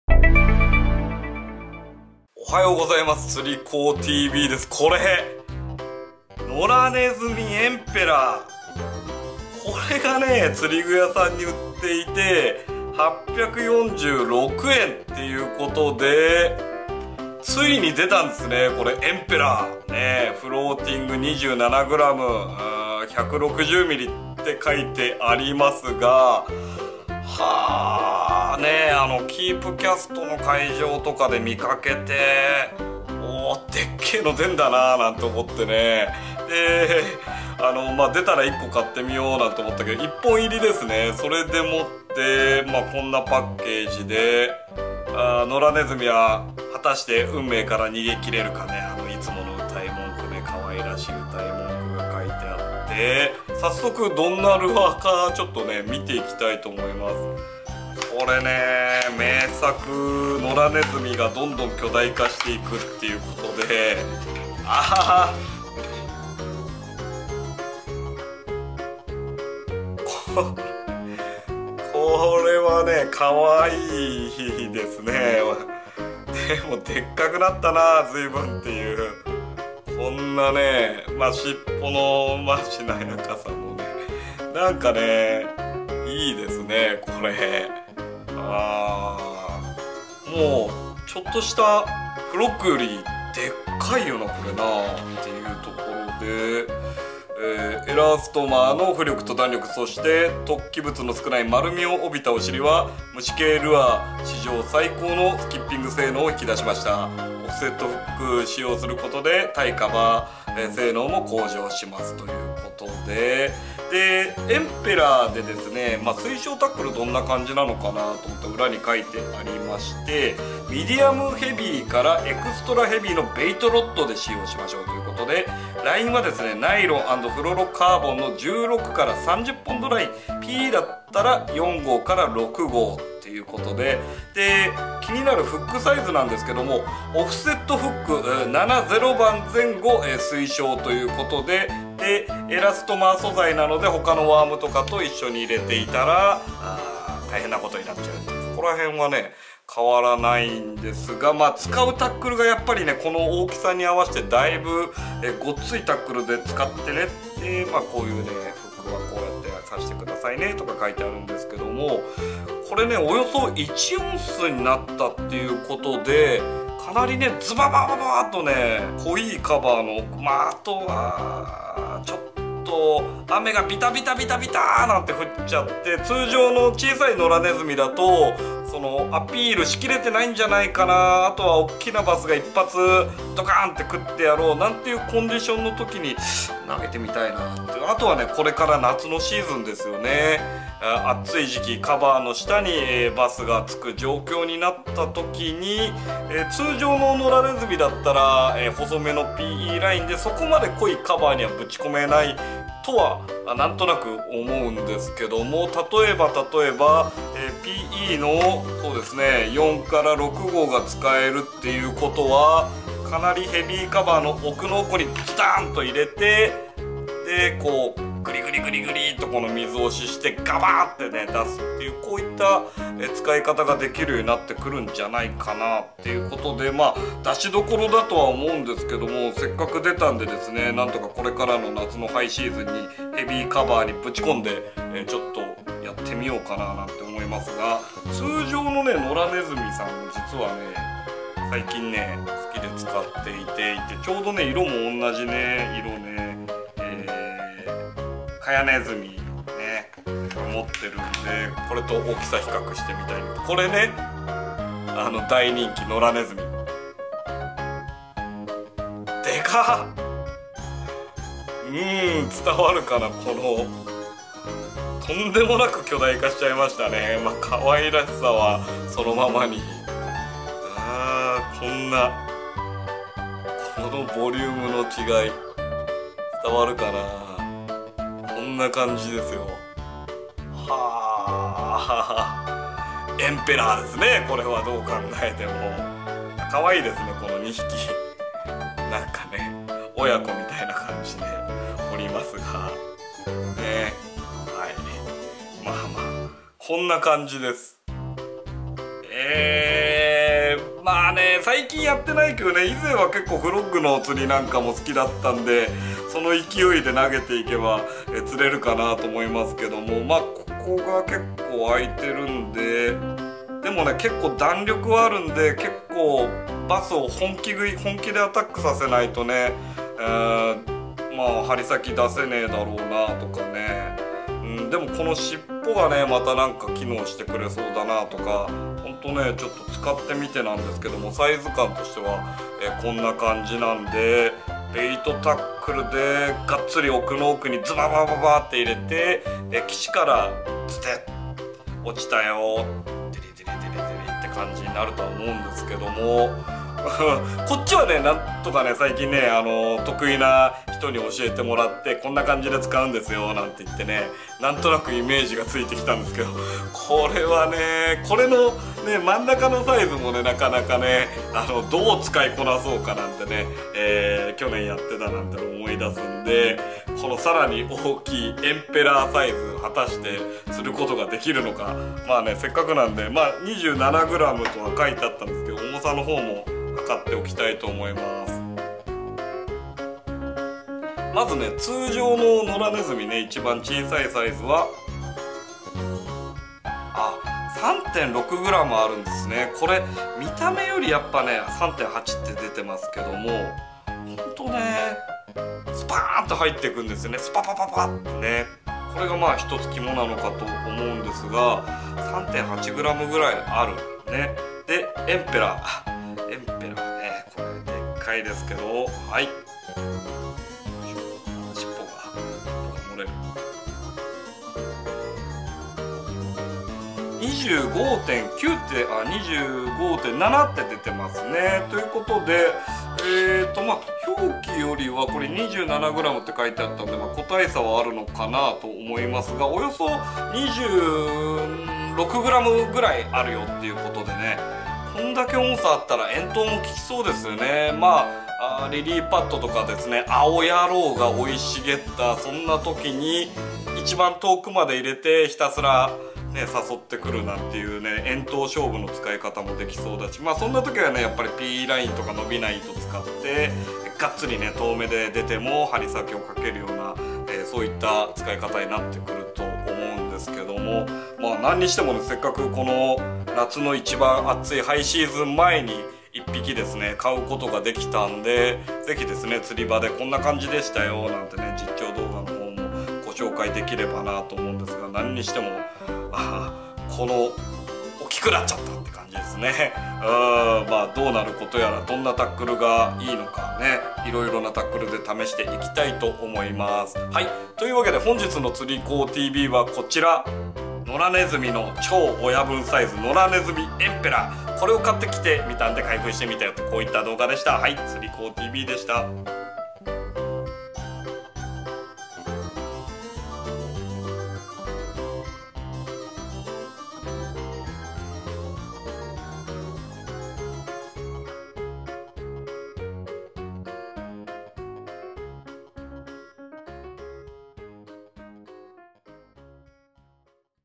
野良ネズミエンペラーが巨大過ぎて困惑
ＳＵＢ！野良ネズミエンペラーが巨大過ぎて困惑って事で人気の野良ネズミに最大サイズのエンペラーが仲間入りです！

2019年度は引き続き去年デプスの「コンツアージグ」＆「ブルフラット」で練習したジグストの釣りを色々なルアーに応用して巨大なブラックバスを狙って行きます！ライトリグ方面では去年よりゲーリーヤマモト大好きな釣りガール、メグタスが加わったのでそれぞれ分担して色々なルアー、釣り方に挑戦して行きますよ〜！

TurikouTVの動画
19アンタレスに真紅のスコーピオン⁉︎シマノの2019最新リールが遂に情報解禁！
19アンタレスはアンタレスミニ⁉︎ シマノの総合カタログが素敵過ぎる